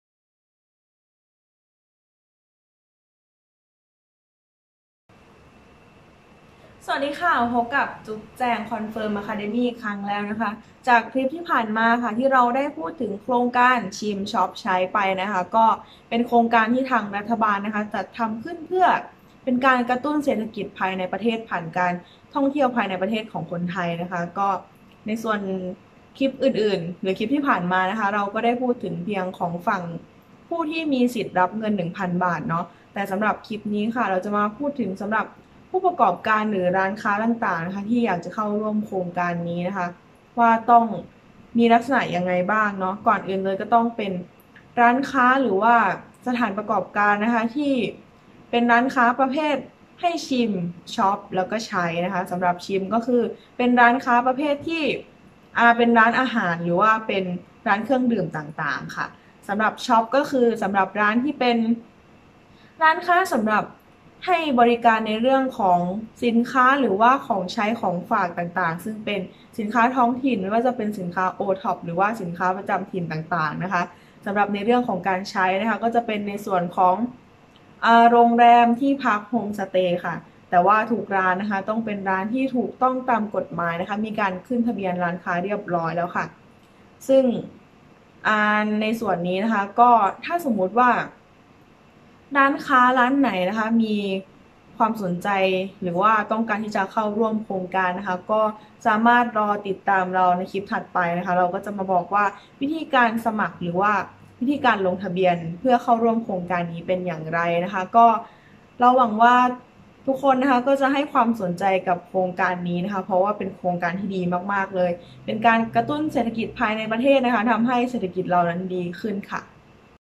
ชิม ช้อป ใช้ – ผู้ประกอบการมีสิทธิเข้าร่วมโครงการ ชิมช้อปใช้
ชิม ช้อป ใช้ – ผู้ประกอบการและร้านค้าที่มีสิทธิเข้าร่วมโครงการชิม ช้อป ใช้

โครงการ ‘ชิม ช้อป ใช้’ เป็นโครงการที่มาจากการเล็งเห็นความสำคัญของการกระตุ้นเศรษฐกิจด้วยการท่องเที่ยวภายในประเทศ โดยทางรัฐบาลจะมอบเงิน 1,000 บาทให้กับคนไทย 10,000,000 คนทั่วประเทศเพื่อไปใช้จ่ายท่องเที่ยวภายในประเทศไทย ซึ่งผู้ที่จะมีสิทธิรับเงินนั้นต้องผ่านการลงทะเบียนเสียก่อน แต่ไม่ใช่แค่ผู้บริโภคเท่านั้น ร้านค้าหรือผู้ประกอบการต่าง ๆ ที่จะเข้าร่วมโครงการก็ต้องผ่านการลงทะเบียนด้วย และต้องเป็นร้านค้าหรือสถานประกอบการที่ถูกต้องตามกฎหมาย ซึ่งลักษณะของร้านค้าที่สามารถเข้าร่วมโครงการได้นั้นคือร้านค้าประเภทชิม ช้อป ใช้ ร้านธงฟ้าประชารัฐ และร้านค้าทั่วไปที่มีแอพพลิเคชั่น ‘ถุงเงิน’ เพื่อใช้ในการรับเงินจากนักท่องเที่ยวต่อไป